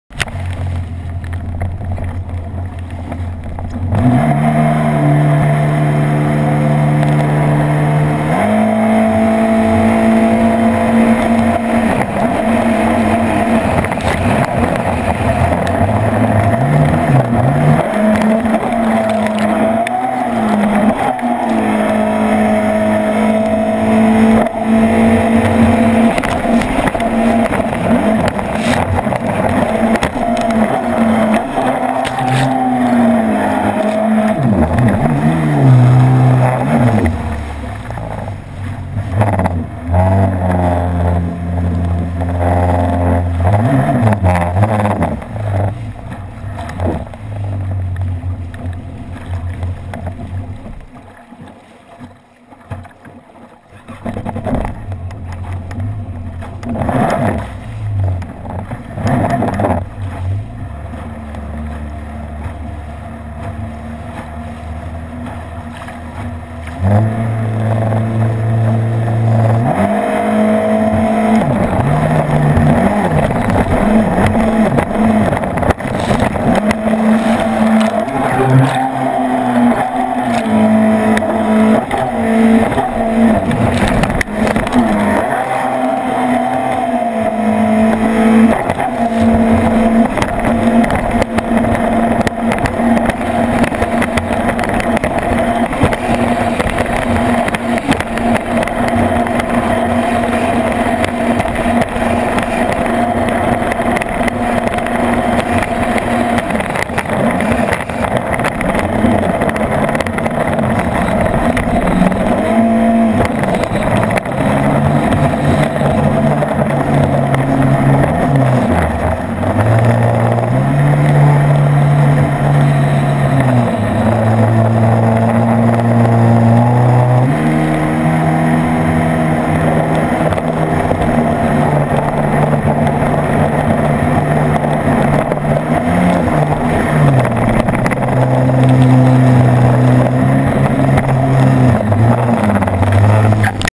Yamaha VXR 2012 - Go Pro Hero2 - Fun
Désolé pour la qualitée d'image.. j'avais installé le devant de la caméra trop en l'air, donc l'eau restait sur la lentille.. prochain coup ca sera mieux!!! (pis dire que j'ai pris le temps de mettre du RainX).. ! :S